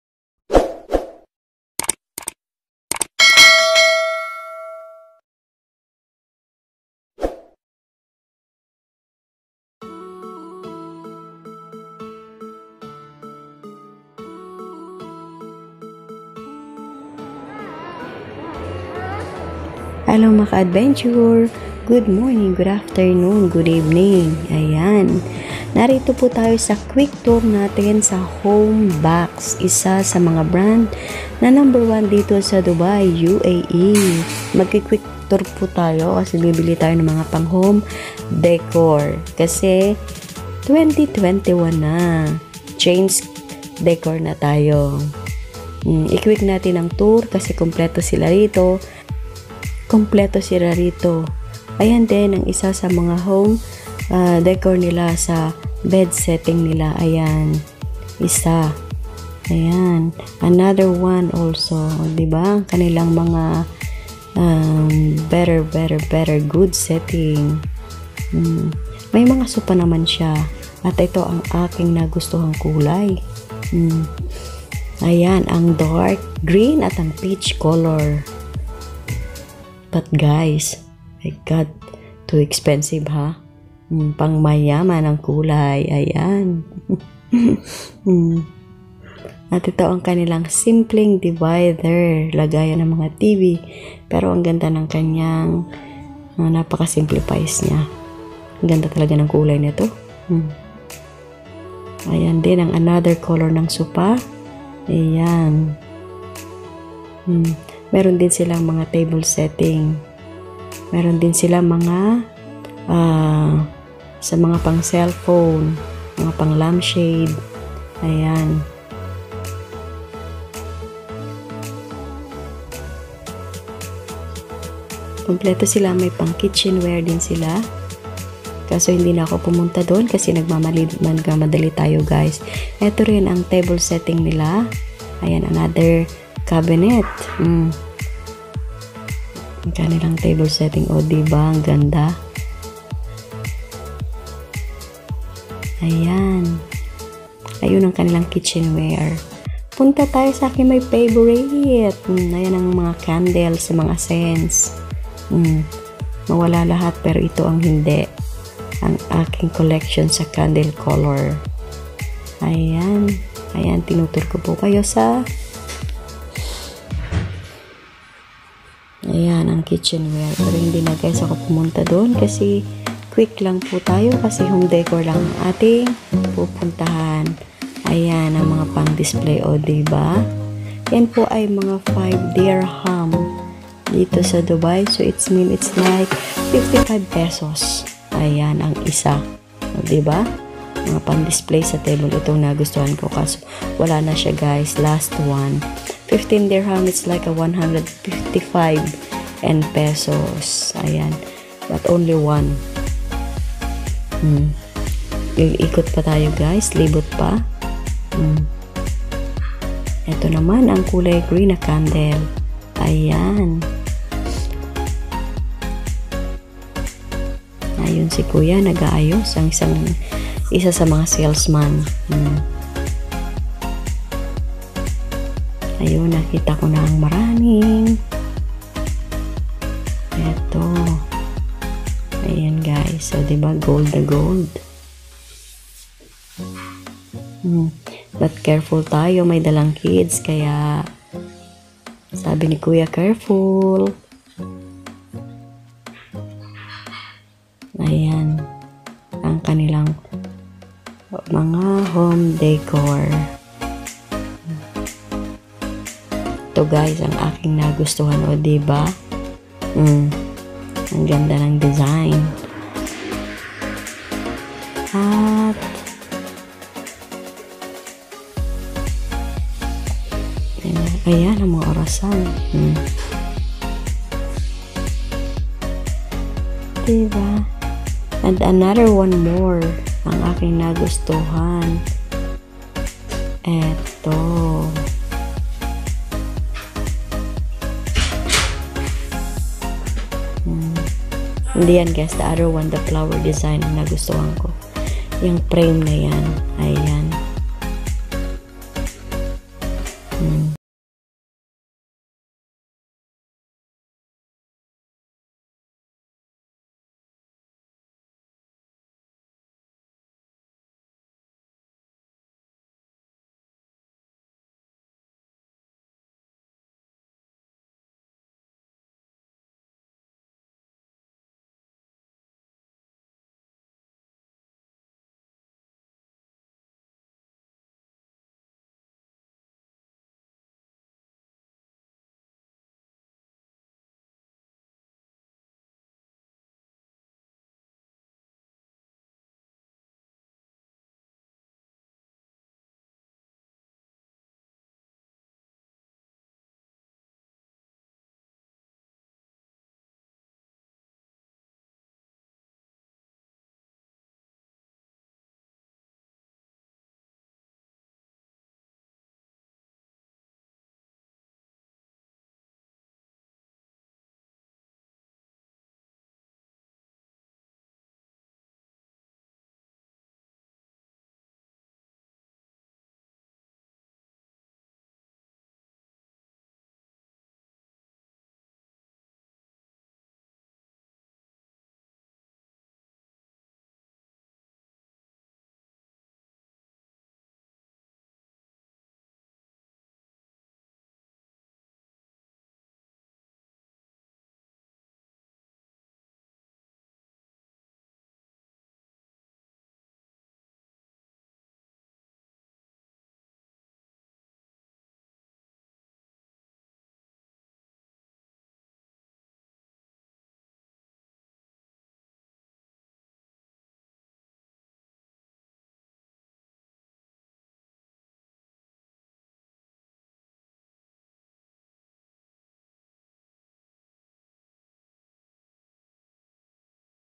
SHOPPING IN HOMEBOX QUICK TOUR
shopping haul and quick tour in homebox . complete in term of home accessories and wonderful item only in home box . it's a lot of branches but which I quick and buy some decor ,
located in ibn battuta mall.
homeboxibnbattutamall